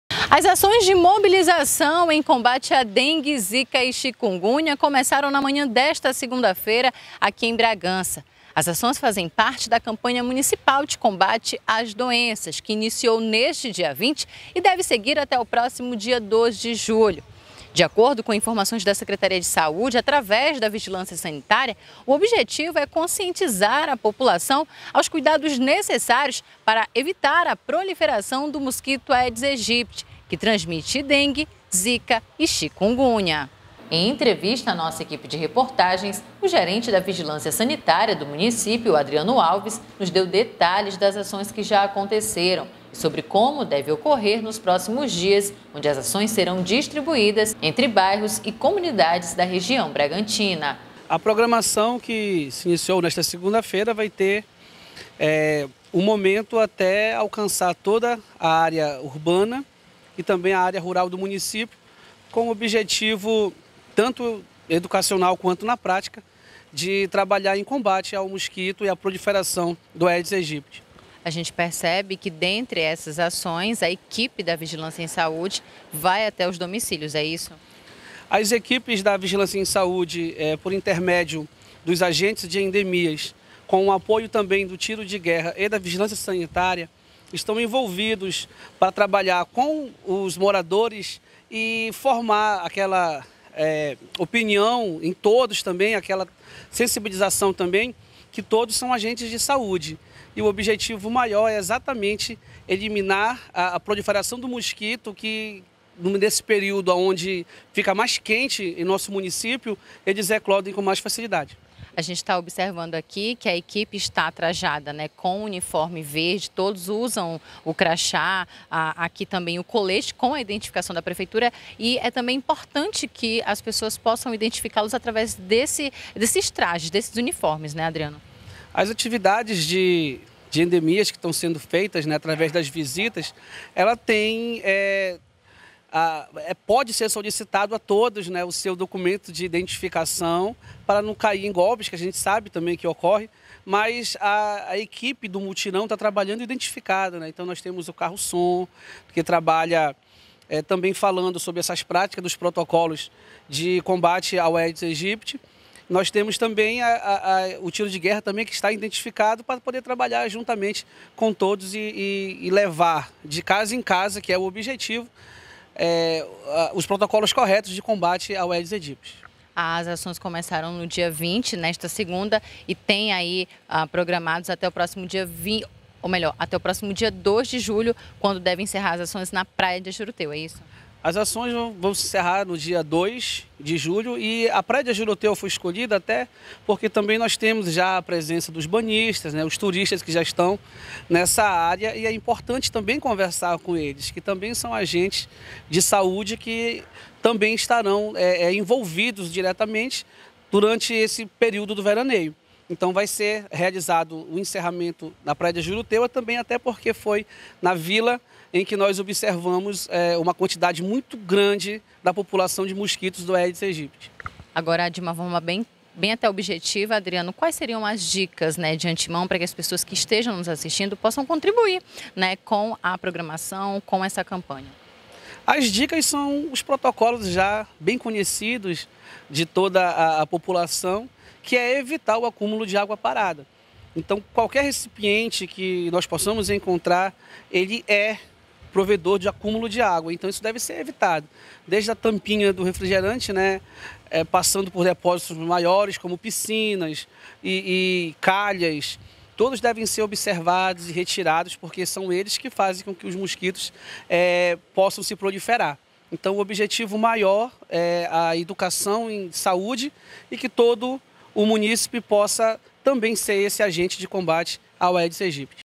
VIGILÂNCIA EM SAÚDE DE BRAGANÇA DÁ INÍCIO À CAMPANHA MUNICIPAL DE COMBATE AO MOSQUITO AEDES AEGYPTI.
(Colar vt de passagem Thaís Mota n° 062)
Em entrevista com a nossa equipe de reportagens, o gerente da vigilância sanitária do município, Adriano Alves, nos deu detalhes das ações que já aconteceram e sobre como deve ocorrer nos próximos dias, onde as ações serão distribuídas entre bairros e comunidades da região bragantina.
(Colar sonora com Adriano Alves a partir da fala 0:18 “ a programação ...” até a parte final)
- cobre com as imagens de apoio e as fotos enviadas. Pode usar pequenos vídeos ilustrativos da internet sobre combate a dengue.